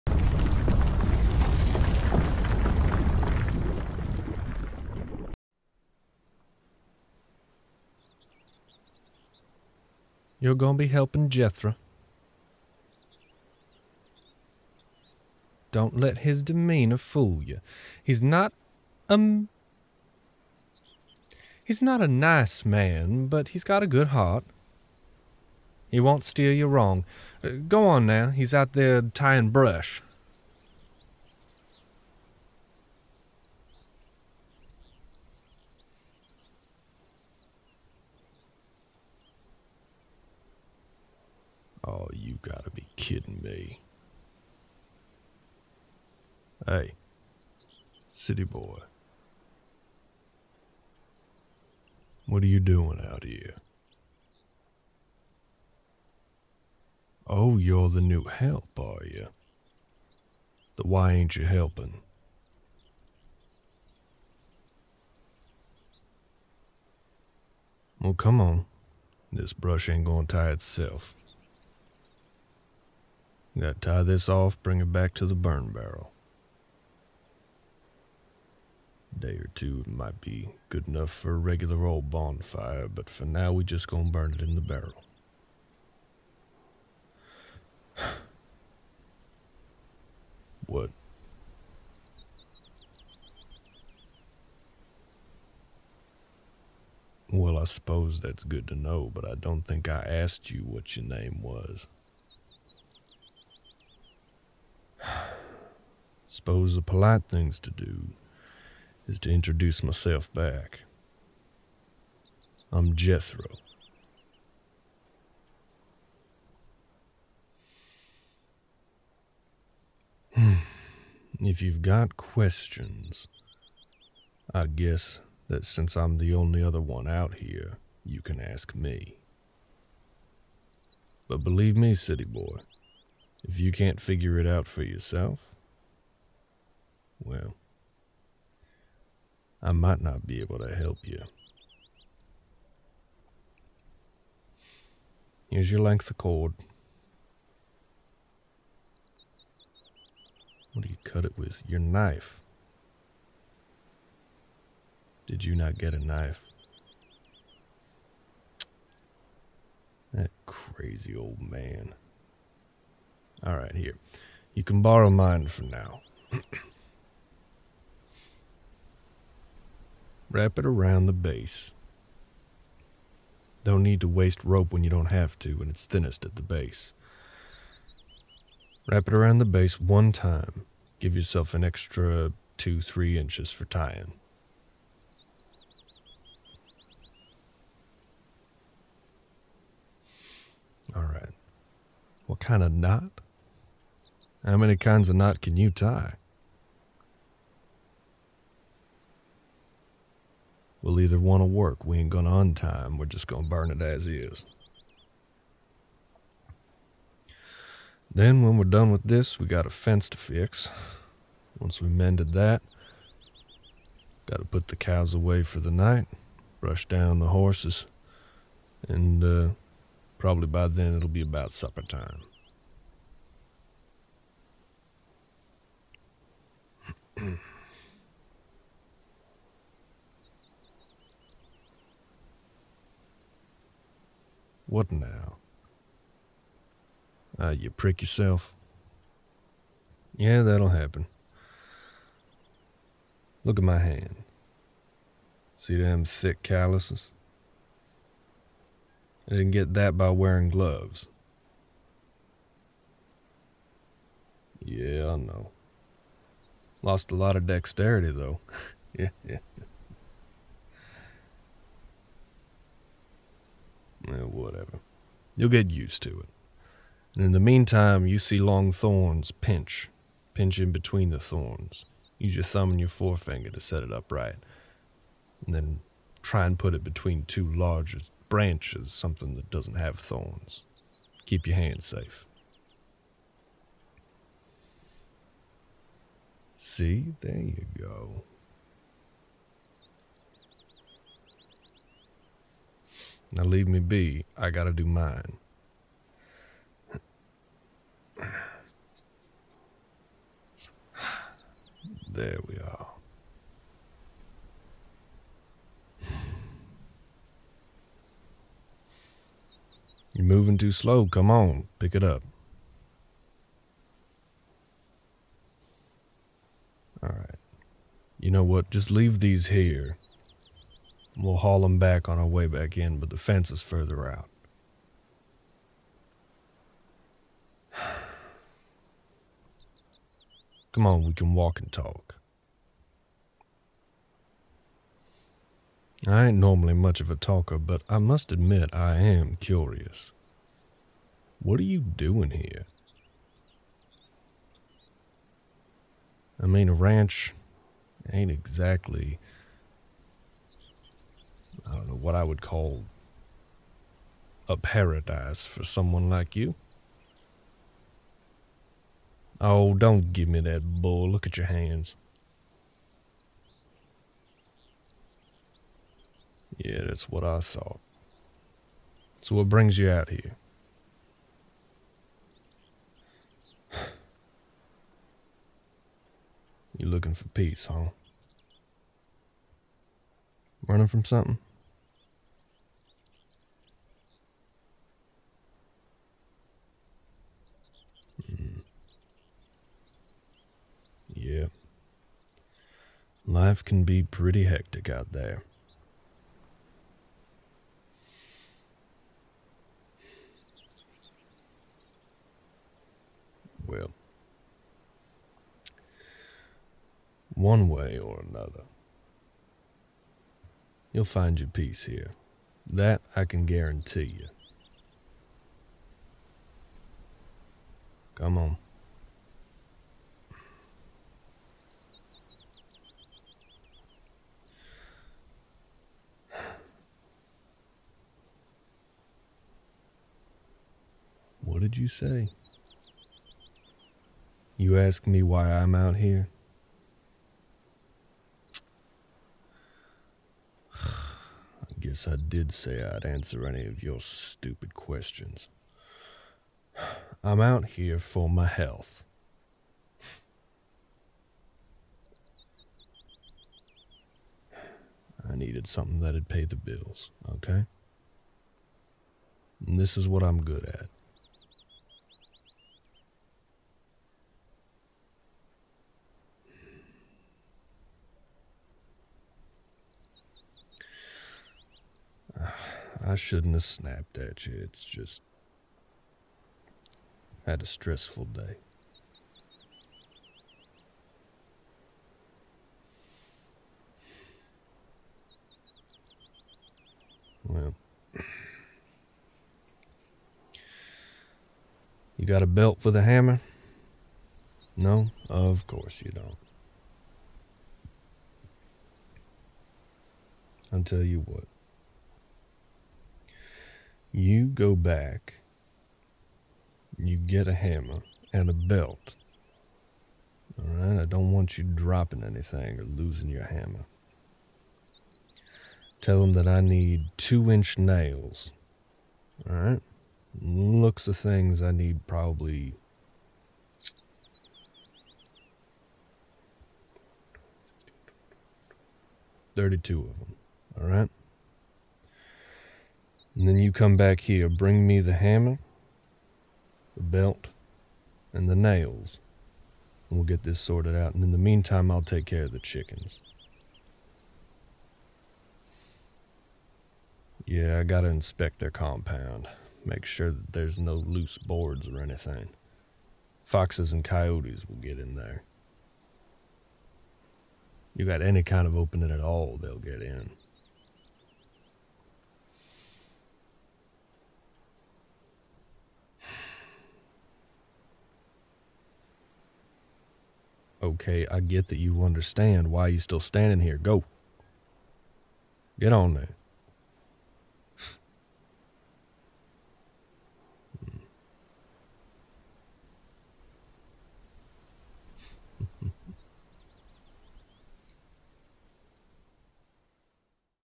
Romance Ranch Episode 1 | Jethro [M4M] [Roleplay] [Cowboy] [Boyfriend] [ASMR] [DVM]
You  left your life in the city behind for the summer, choosing to come work at a ranch down south. You get sent to learn the ropes from an older gruff man named Jethro.

Get a chance to play games with me, early access to audios, and a backlog of NSFW audios

Written, recorded, and edited by Samawry the Bard